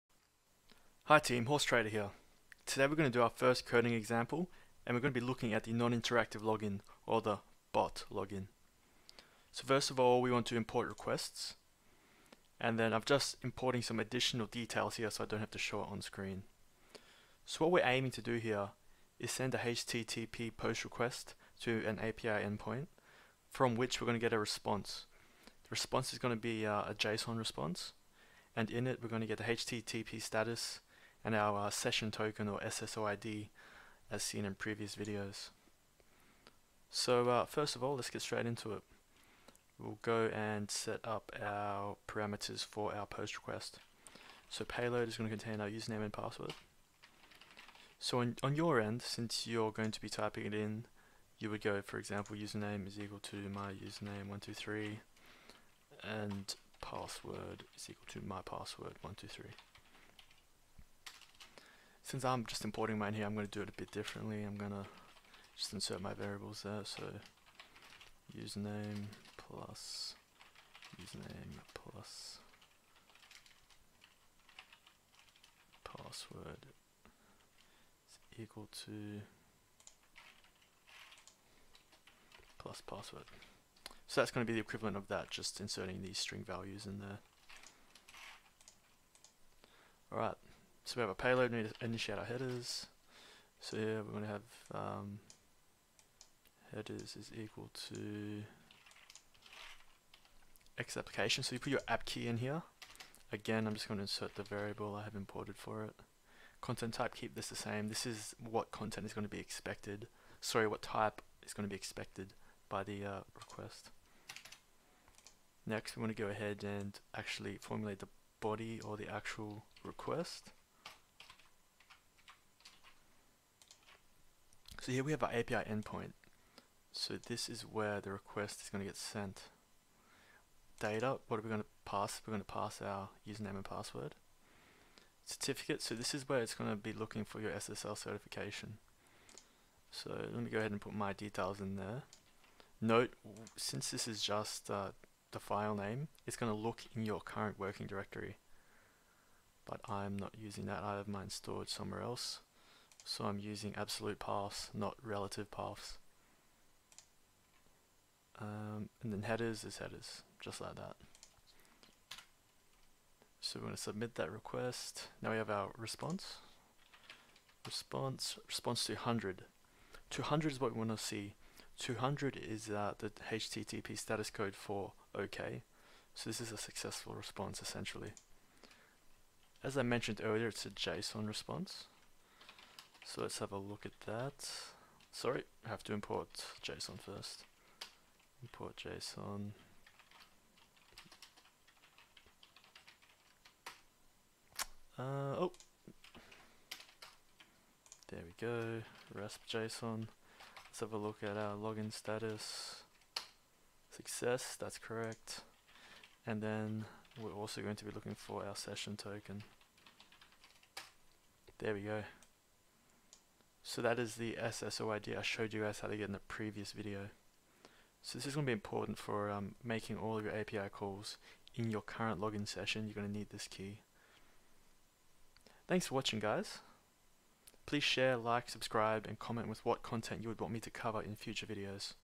BETFAIR API - Non Interactive Login - Python Coding
Hi Guys,

In this video we are going to explore a python coding implementation of the non-interactive (bot) login.